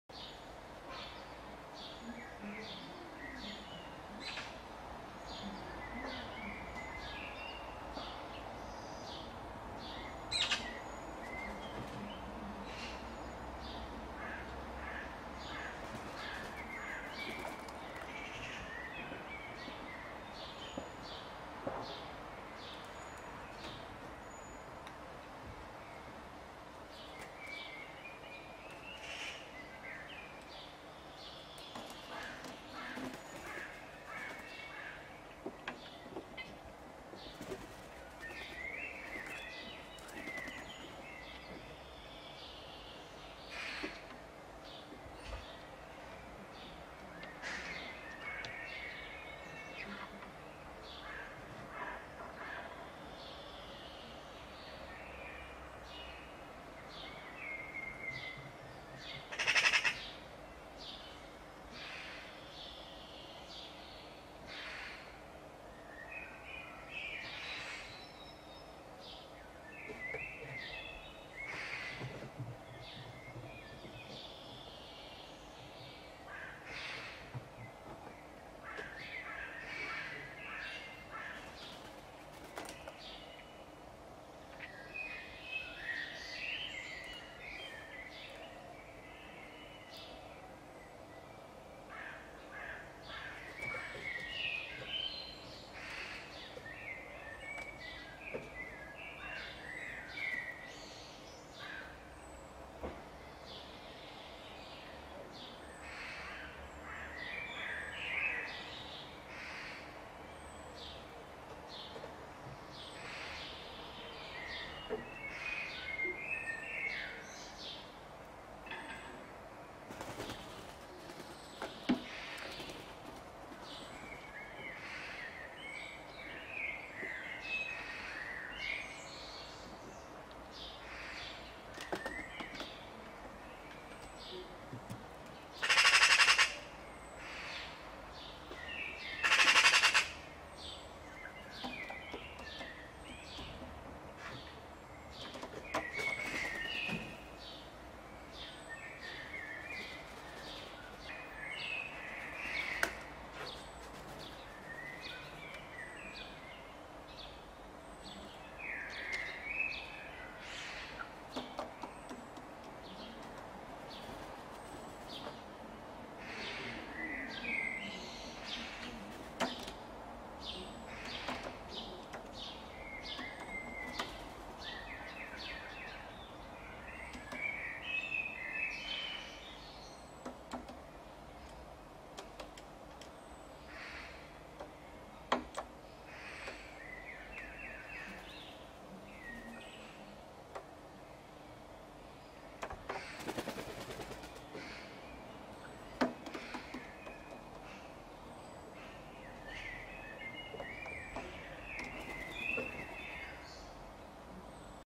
KATER vs ELSTERN....wer bekommt das Futter?
Unser Kater hatte es sich neben dem Futter bequem gemacht. Allerdings gibt es da ein paar
Störenfiede...